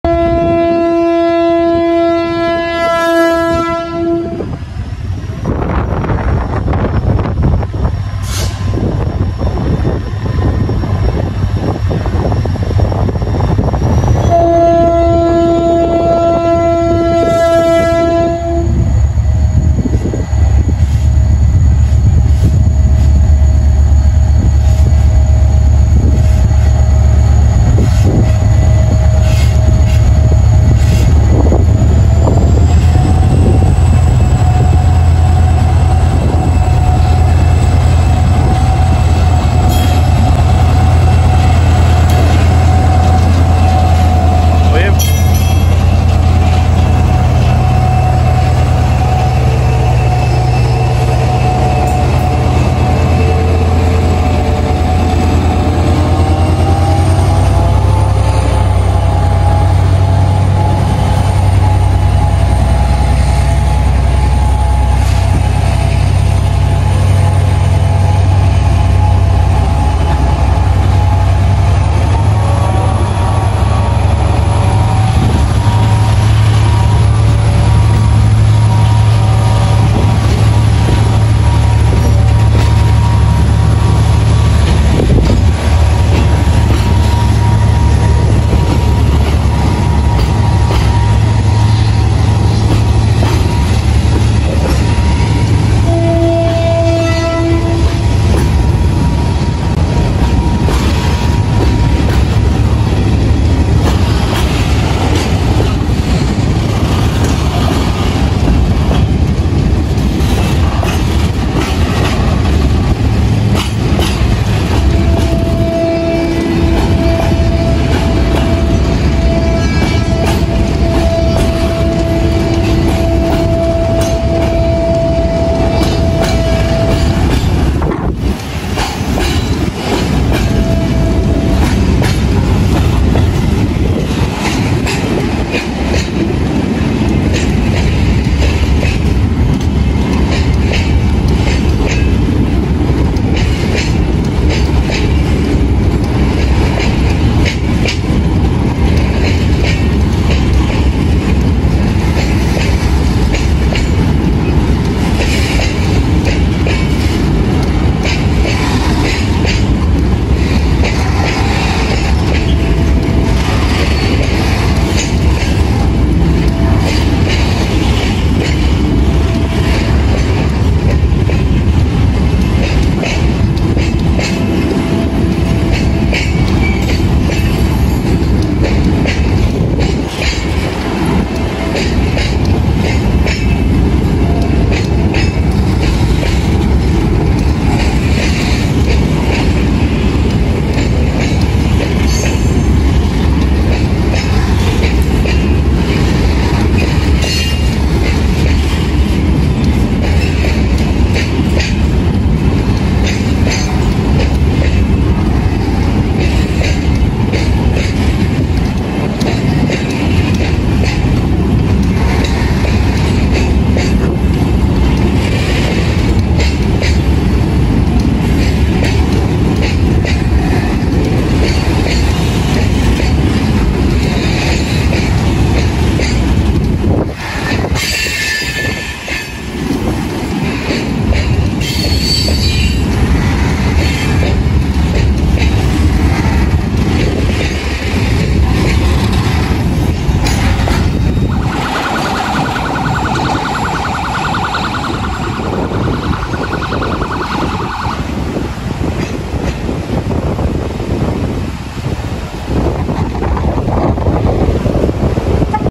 GT22 9058 ESPECTACULAR salida de LAS FLORES | Ferrosur Roca. Marzo 2023
Recién llegados a la ciudad de Las Flores nos encontramos con este largo tren cementero de Ferrosur esperando para salir hacía Cañuelas. Con la 9058 a la cabeza, el cementero sale al Ochazo de la estación.